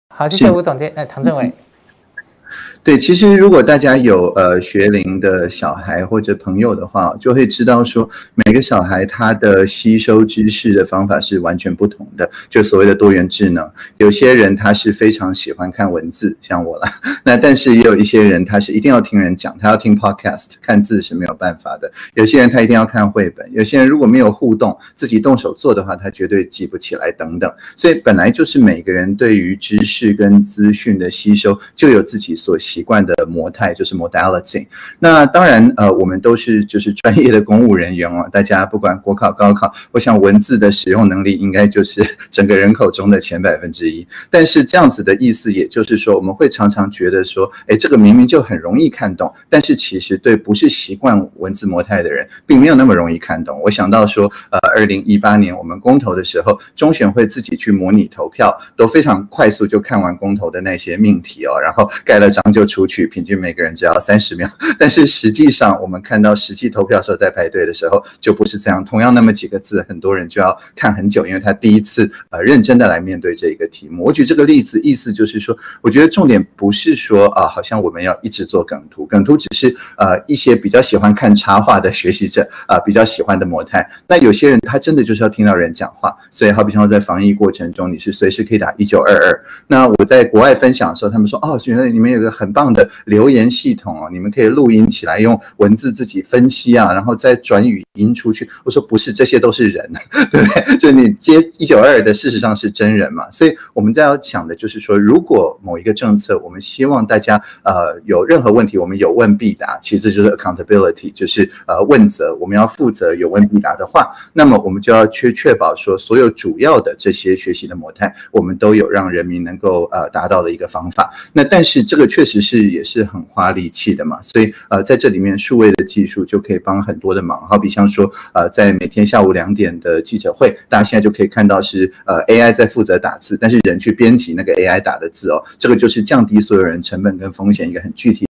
[唐鳳演講]民眾看不懂，政策要如何進行下去?
政府如果一直怪民眾看不懂不會用，沒辦法吸引民眾去主動去瞭解，這些政策要如何進行下去?
知識與資訊的媒介
公投
梗圖
ex防疫記者會AI文字輸入